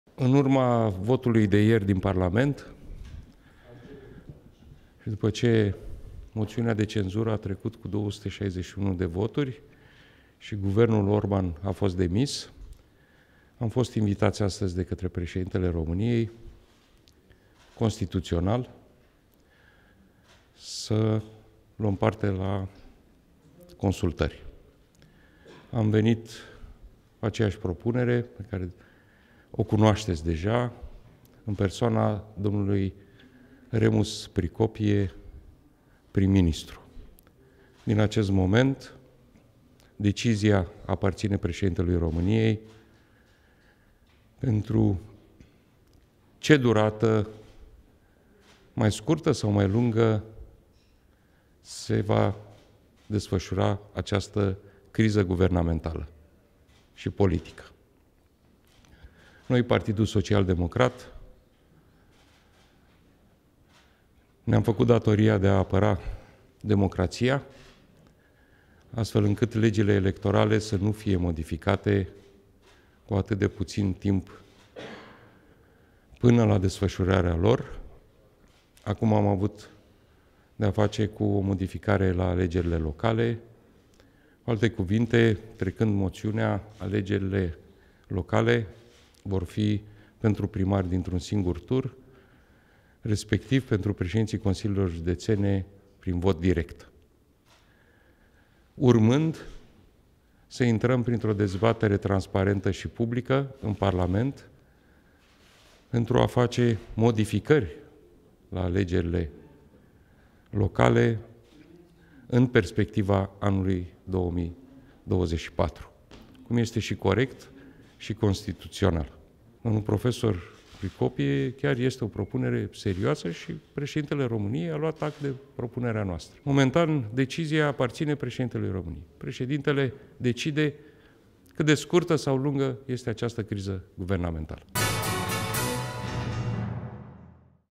Consultări la Cotroceni/Ciolacu: Am venit cu propunerea Remus Pricopie - prim-ministru
Preşedintele interimar al PSD, Marcel Ciolacu, a anunţat joi, după consultările de la Palatul Cotroceni, că i-a prezentat preşedintelui Klaus Iohannis propunerea de a-l desemna premier pe Remus Pricopie.